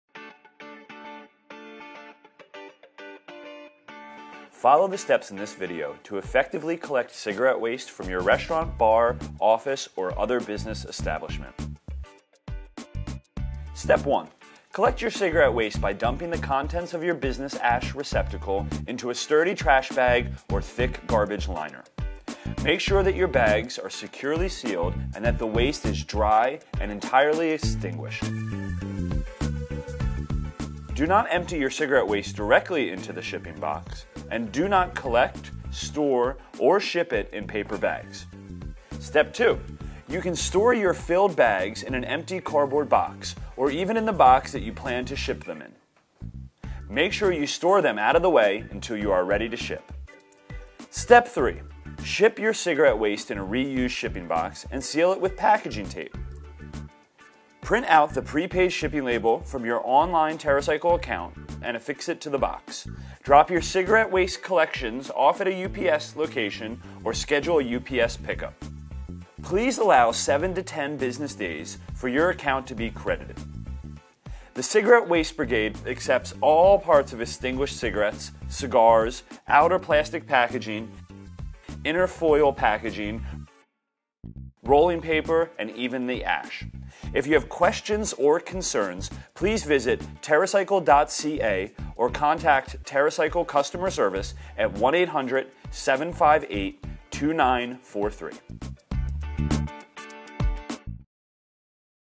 Cigarette butt Waste recycling - TerraCycle
How to collect for the Cigarette Waste Brigade at your office, restaurant, or other business establishment.

It is shared on this channel for informative purposes only, found in this post